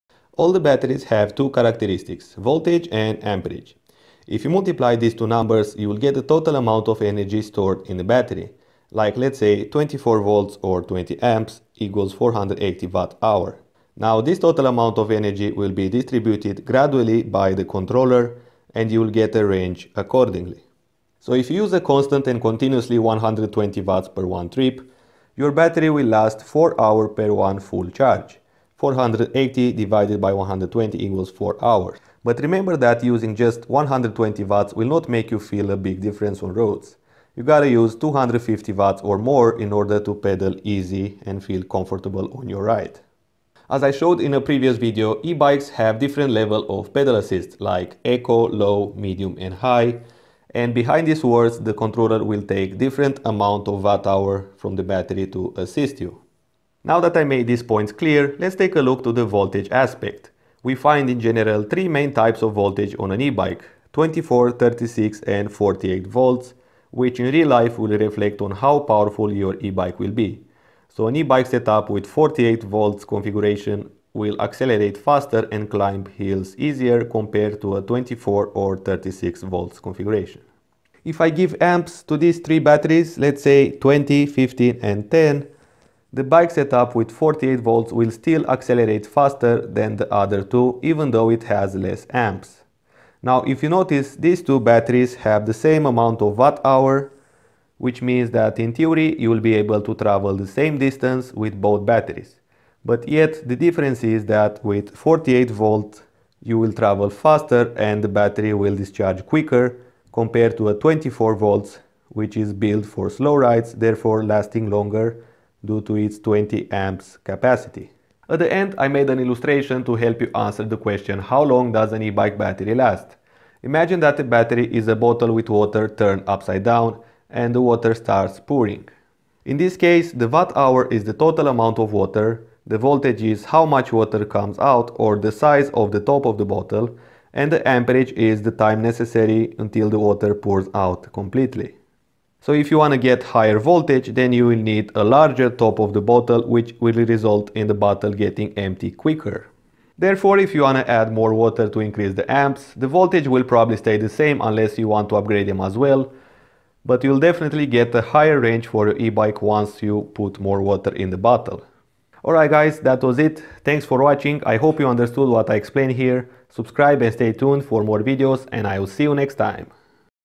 What is the Range of an E -bike Battery ? EXPLAINED Part 2
how long does an e bike battery last ? how far you can travel with one full charge ? how to chose a battery for your needs ? What is the range of an e bike ?